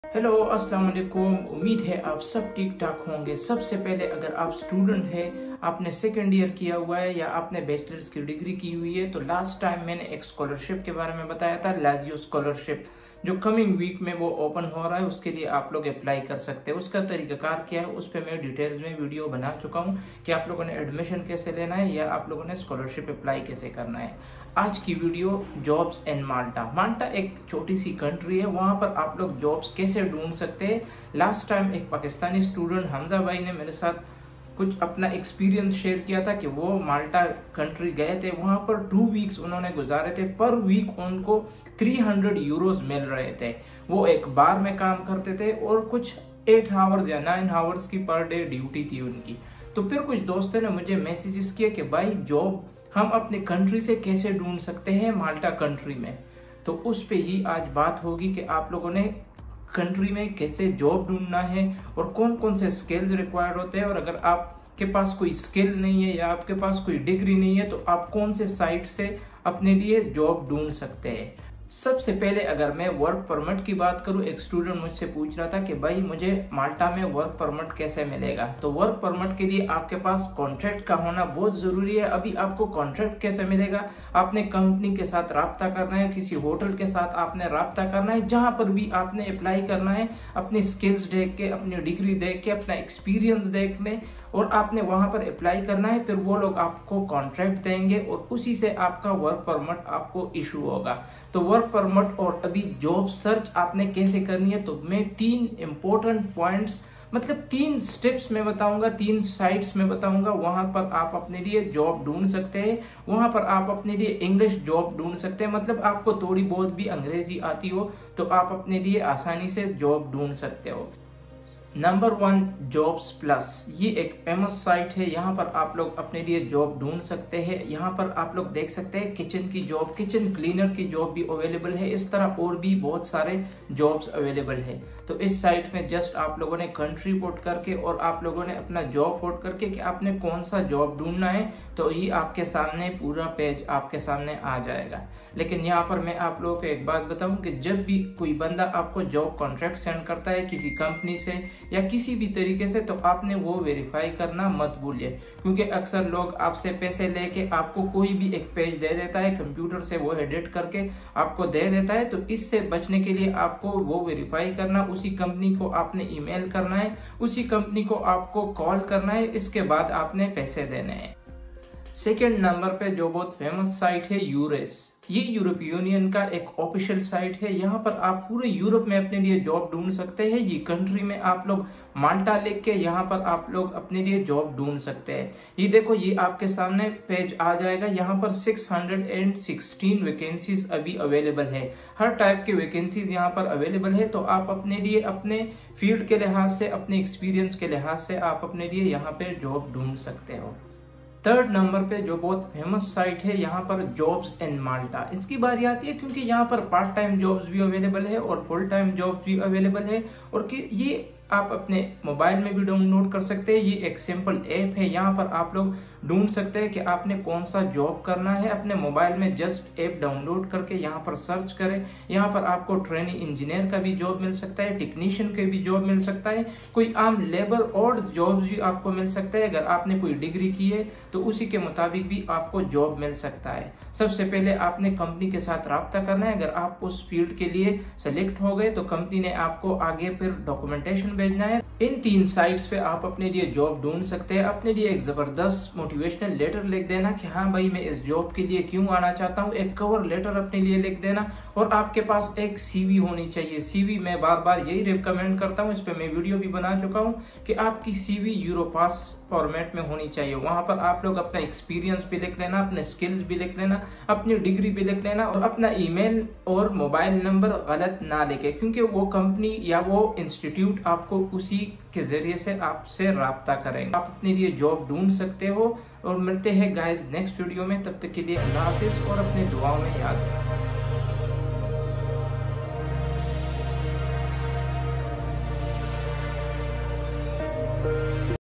Jobs in Malta || How to find work in Malta || Online Job search
In this video, I talk about the Jobs in Malta that how we will find jobs in Malta from our country.
So I talk about three famous sites where we will easily learn how to find work in Malta as part time or full-time job depending upon our skills as well as degree. It teaches how you can do an Online job search on these websites which are given below.

These three are famous websites for finding work in Malta.